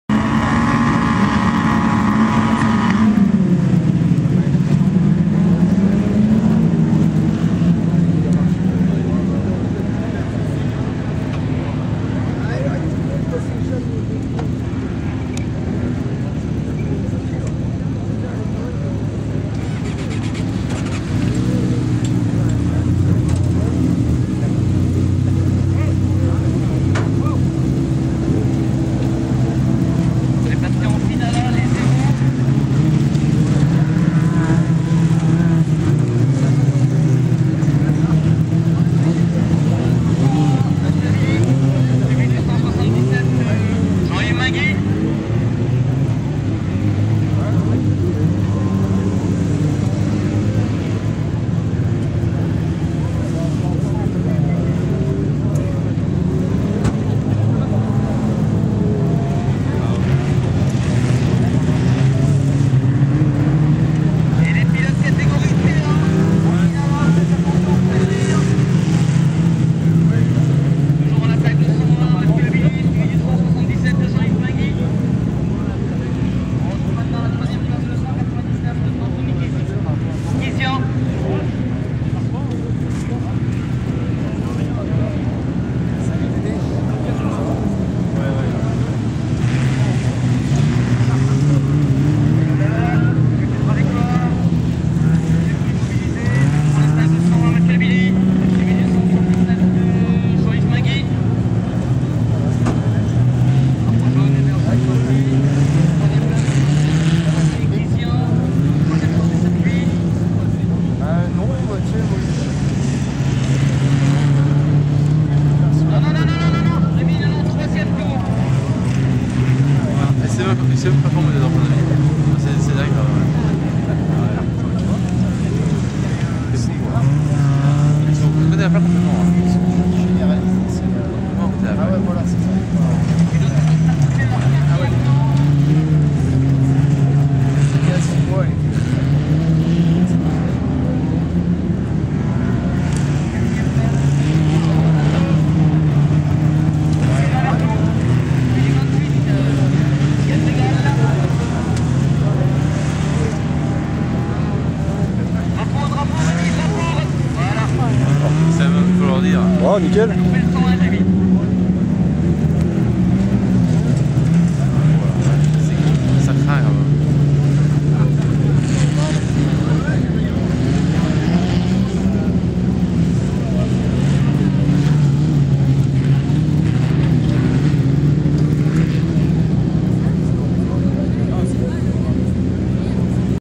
finale kart évo Sizun 2013
Auto-cross de Sizun (29) le 28/4/2013 finale Kart évo avec moteur d'AX 1124 cm3